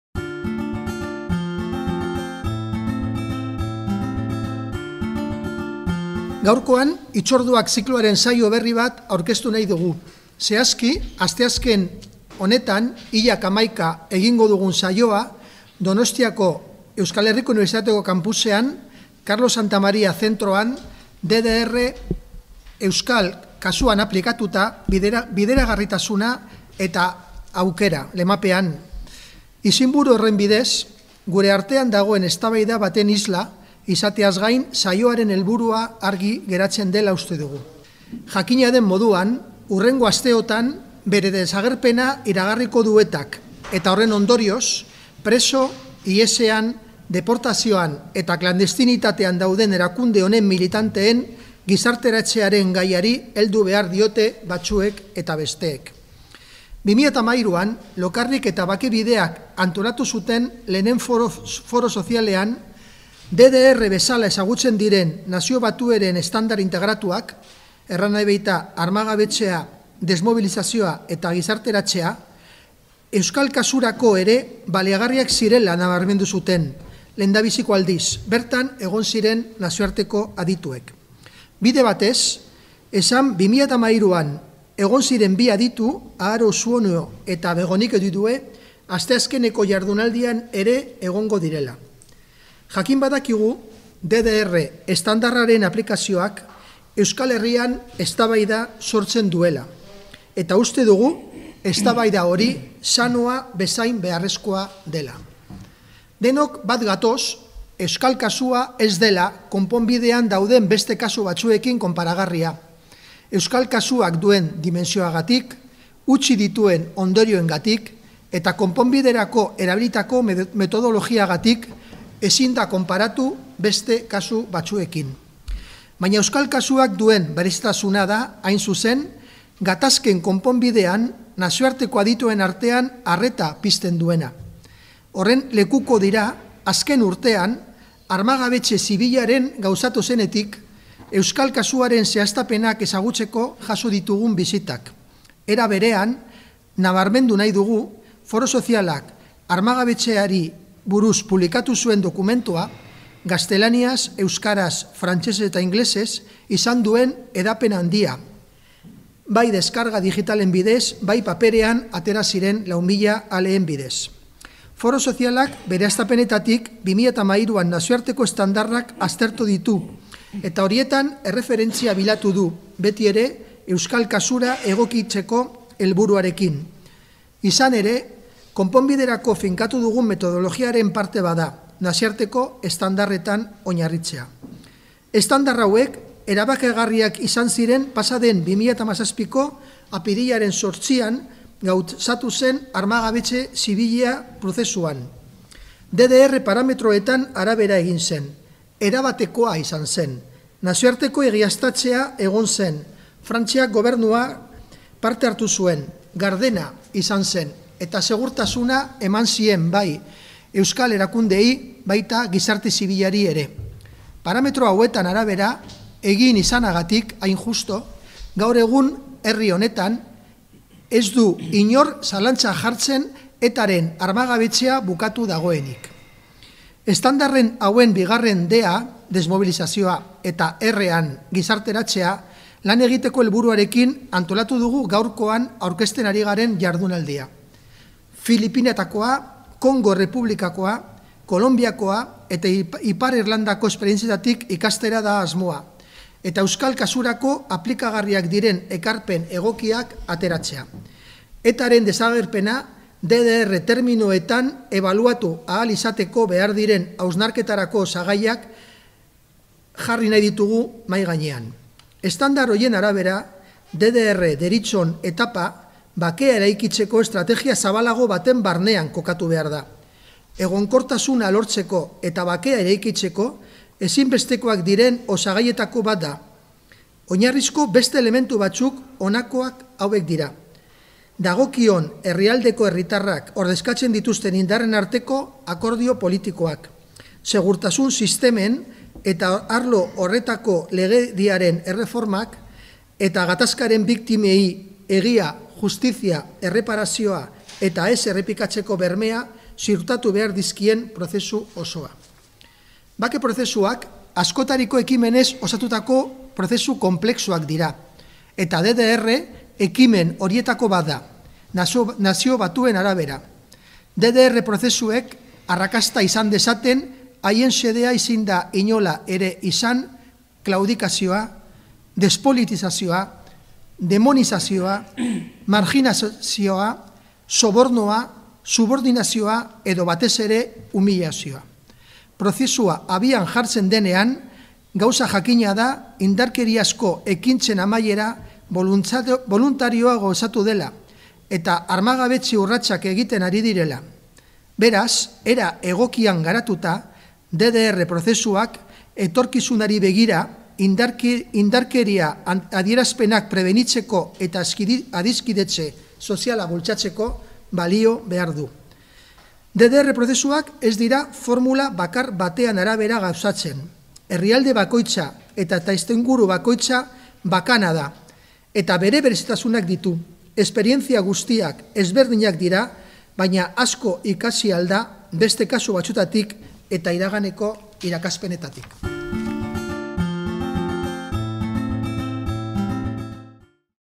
Rueda prensa "El DDR aplicado al caso vasco: viabilidad y oportunidad"
Foro Sozial Iraunkorrak (Bakea eta Duintasunarekin lankidetzan) hurrengo Hitzorduan parte-hartzeko gonbidapena luzatzen dizue.  Hitzorduaren izenburua "DDRa (Desarmea, Desmobilizazioa eta Bergizartzeratzea) euskal kasuan: bideragarritasuna eta aukera" izango da eta Kolonbia, Filipinak, Ipar Irlanda eta Kongoko Errepublika Demokratikoko bake prozesuetako ikasketak hurbiltzeko asmoa izango du.

Foro Sozial Iraunkorreko kidea den Bakea eta Duintasuna Elkarteak antolatutako hitzordu hau, apirilaren 11n (asteazkena) burutuko da 16:30etatik aurrera, Carlos Santamaría Zentroko Antonio Beristain Auditorioan (UPV/EHU - Donostia).

Desde el Foro Social Permanente ( en colaboración con Paz con Dignidad) os invitamos a participar en el próximo Hitzorduak, que con el título "El DDR (Desarme, Desmovilización, Reintegración) aplicado al caso vasco: viabilidad y oportunidad" pretende acercarnos aprendizajes de otros procesos de paz como los de Colombia, Filipinas, Irlanda del Norte o República Democrática del Congo.

Este Hitzorduak organizado por la Asociación Paz con Dignidad, organización integrante del Foro Social Permanente, se celebrará el 11 de abril (miércoles) a partir de las 16:30 en el Auditorio Antonio Beristain del Centro Carlos Santamaría(UPV/EHU - Donosti).